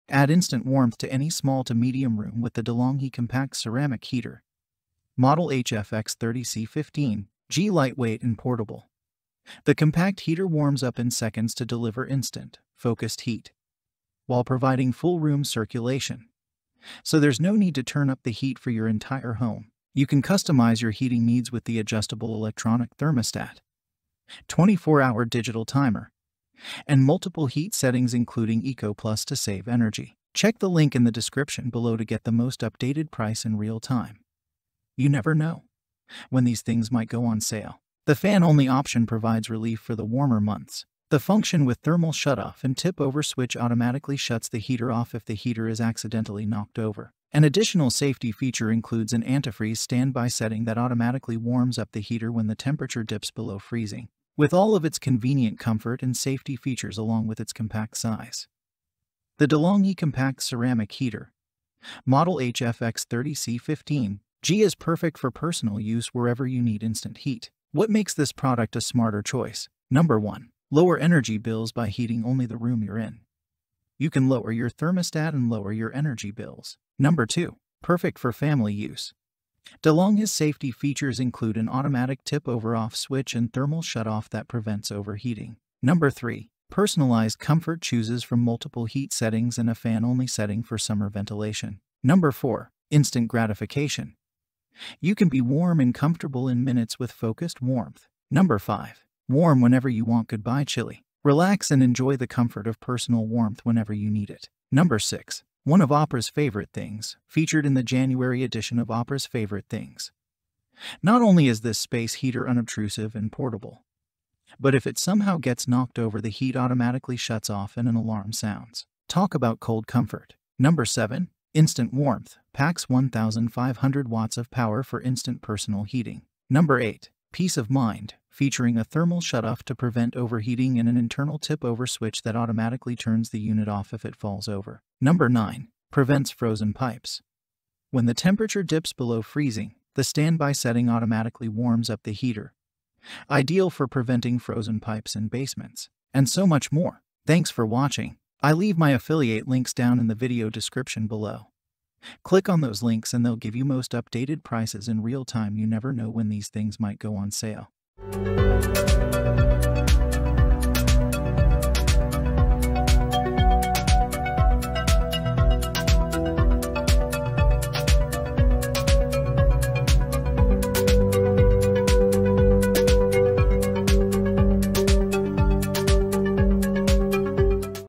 Delonghi HFX30C15.G Capsule Ceramic Heater Review
Check Amazon's latest price (These things might go on Sale)

The De'Longhi Compact Ceramic Heater, model HFX30C15, provides immediate warmth to any small to medium-sized space. The small heater heats up in seconds to provide immediate, concentrated heat while allowing complete room circulation, eliminating the need to crank up the heat throughout your house. The adjustable electronic thermostat, 24-hour digital timer, and various heat settings, including Eco Plus to conserve energy, allow you to tailor your heating requirements.

The fan-only option is useful during the hotter months. If the heater is inadvertently knocked over, the feature with thermal cutoff and tip-over switch immediately turns it off. An antifreeze standby option, which automatically heats up the heater when the temperature falls below freezing, is another safety feature. The De'Longhi Tiny Ceramic Heater, model HFX30C15. G, with all of its practical comfort and safety features, as well as its compact size, is ideal for personal use anywhere immediate heat is required.

Number one, reduce your energy costs by heating just the space you're in. You may do this by lowering your thermostat and lowering your energy bills.

Number 2: De'Longhi's safety features, which are ideal for family usage, include an automated tip-over "off" button and a thermal shut-off that prevents overheating.

Number three, personalised comfort, allows you to choose from various heat settings as well as a fan-only option for summer ventilation.

Number 4: INSTANT GRATIFICATION: With "targeted warmth," you may feel warm and comfortable in minutes.

Number 5: Say goodbye to the cold. Relax and enjoy the convenience of personal warmth whenever it is required.

Sixth, one of Oprah's favourite things: Featured in the January issue of Oprah's Favorite Things: 'Not only is this space heater inconspicuous and portable, but if it is knocked over, the heat automatically turns off and an alert sounds.' Talk about cold solace.

Number 7: Instant Warmth: 1,500 watts of electricity for immediate personal warmth.

Number 8: Peace of mind: It has a thermal cutoff to avoid overheating and an inbuilt tip-over switch that shuts the device off immediately if it falls over.

Number 9: Prevents Frozen Pipes: When the temperature falls below freezing, the standby mode automatically heats up the heater, which is excellent for avoiding frozen pipes in basements.

Searches related to Delonghi HFX30C15.G Capsule Ceramic Heater Review